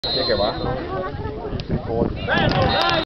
Luciano Manara Casatese 2 - 2
Partita per il ampionato di Promozione tra Luciano Manara e Casatese. L'incontro termina con un pareggio (2-2). Il rigore della Casatese